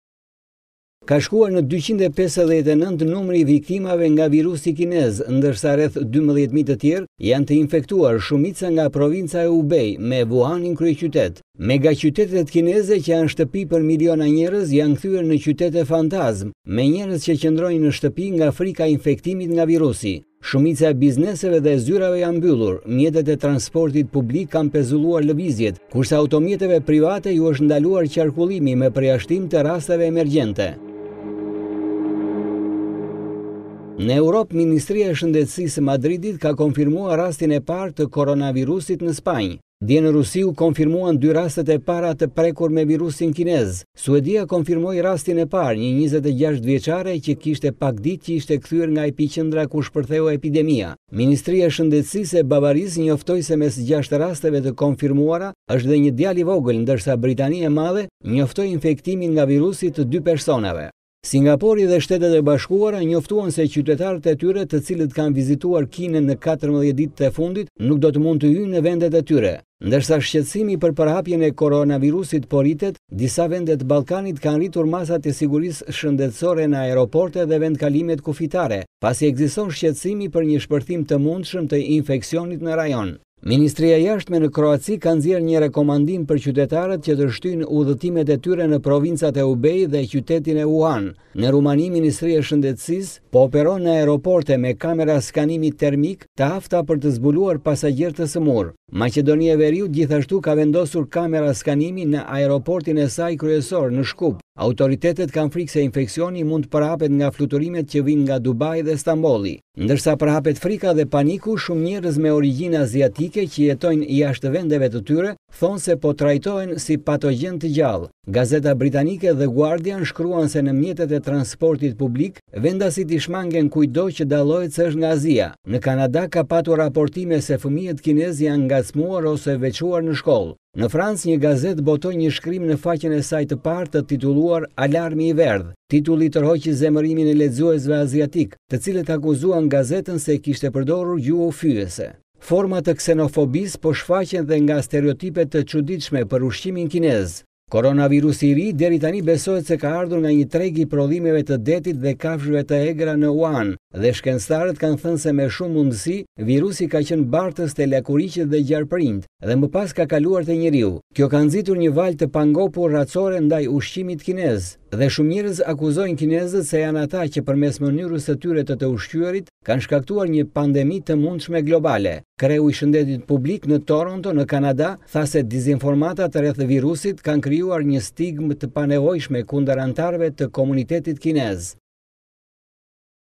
Koronavirusi, rritet në 259 numri i viktimave, numri i të infektuarve rreth 12 000
Ka shkuar 259 numri i viktimave nga koronavirusi kinez ndërsa rreth 12 000 të tjerë janë të infektuar, shumica nga provinca e Hubei, me Wuhanin kryeqytet.

Qytetet kineze që janë shtëpi për miliona njerëz, janë kthyer në qytete fantazmë, me njerëz që qëndrojnë në shtëpi nga frika e infektimit nga virusi.